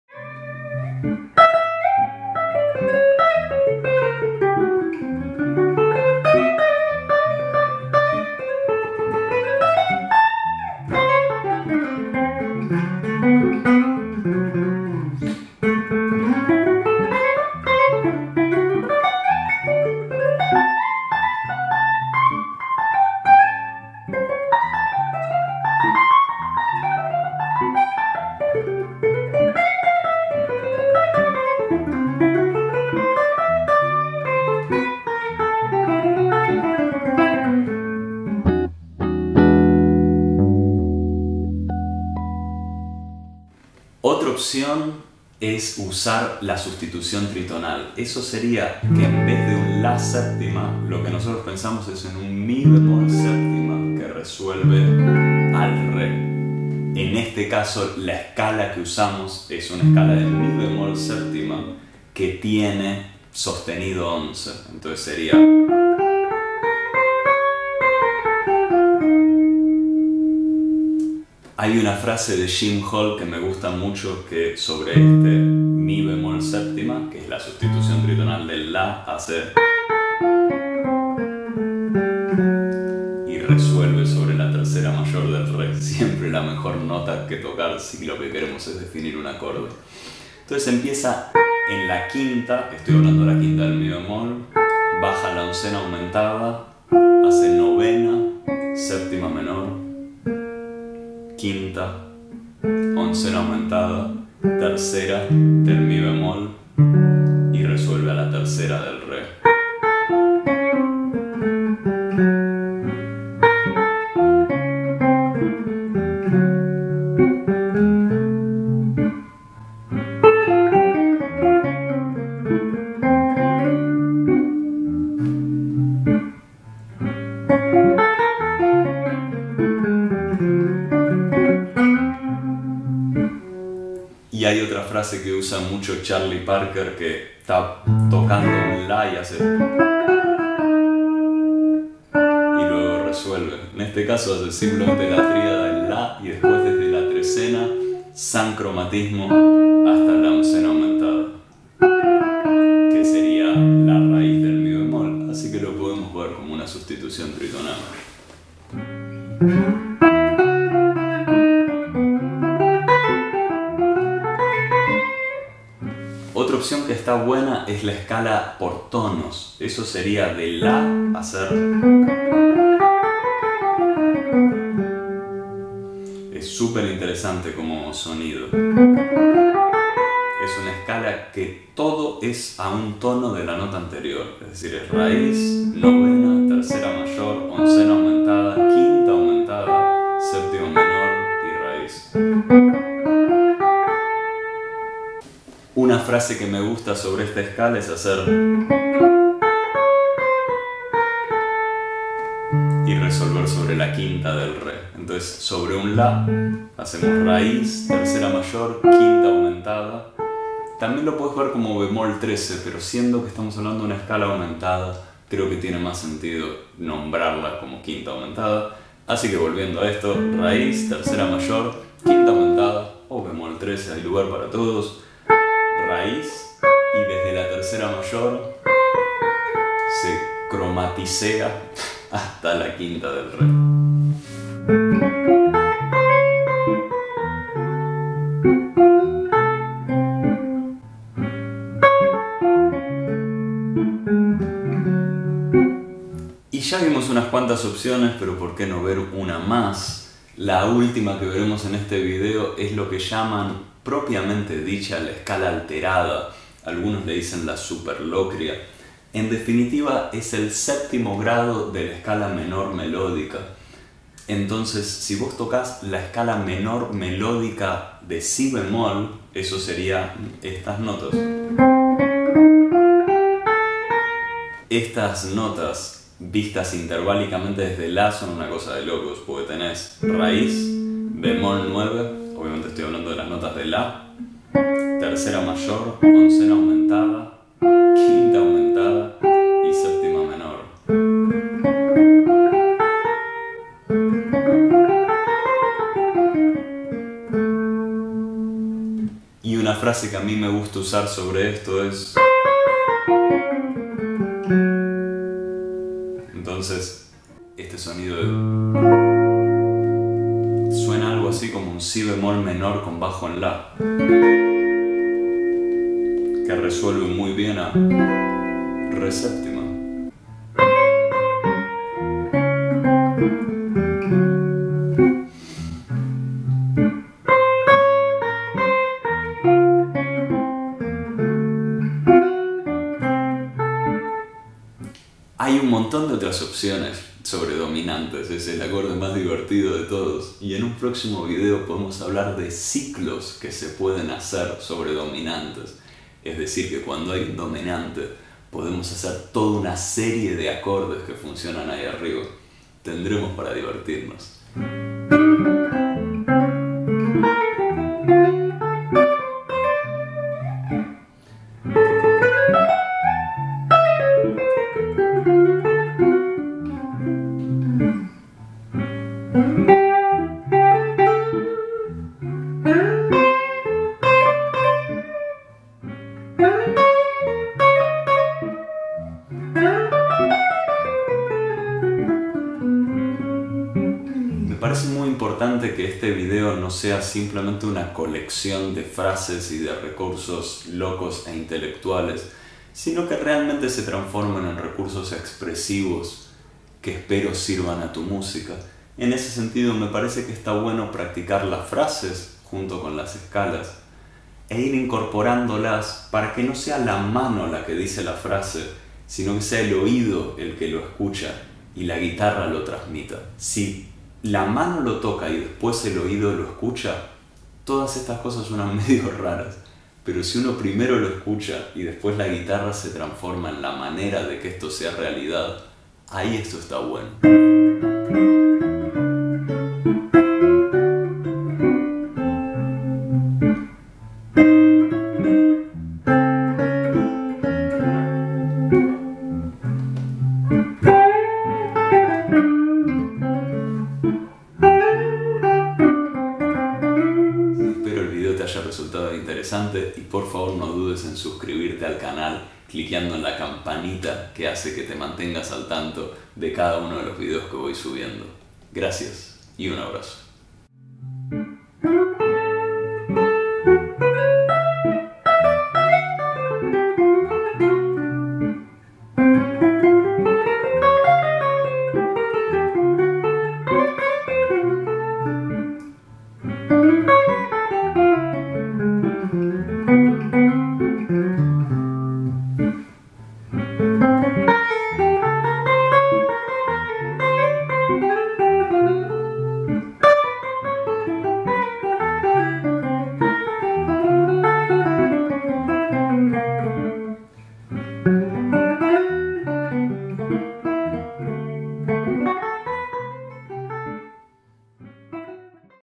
Escalas alteradas sobre dominantes, parte 2 | Pedro Bellora
¿Ya viste la primera parte de este video? En ese caso, es hora de algunas nuevas escalas alternativas para buscar un poco de tensión extra (¡que a veces hace falta!) en esos pícaros dominantes: sustitución tritonal, escala por tonos y escala alterada.